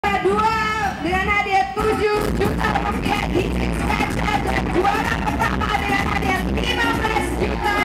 Luigi, Flearock (SKMZ) and Katsu (MZK) wearing Batik
Bboy Luigi (SKMZ)
Bboy Flearock (SKMZ)
Bboy Katsu (MZK, Japan)

They're wearing Batik (Indonesia's traditional cloth) at Hipnotis Flexi Dance Terror 2011 in Indonesia